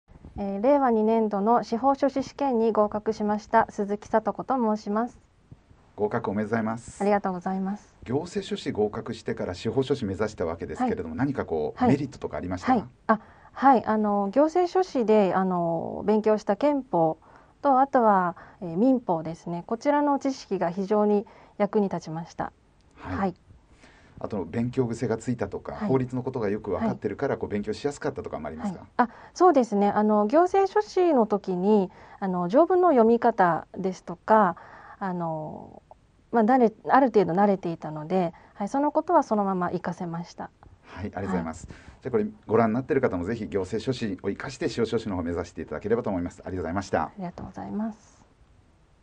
【ＬＥＣ司法書士】令和2年度合格者が語る行政書士からのステップアップ
行政書士試験合格後、司法書士を目指し見事令和2年度司法書士試験に最終合格された鈴木さんに森山講師がインタビュー！

行政書士の学習経験がある方におすすめのコース

☆LEC司法書士の講義を体験！おためしWeb受講制度☆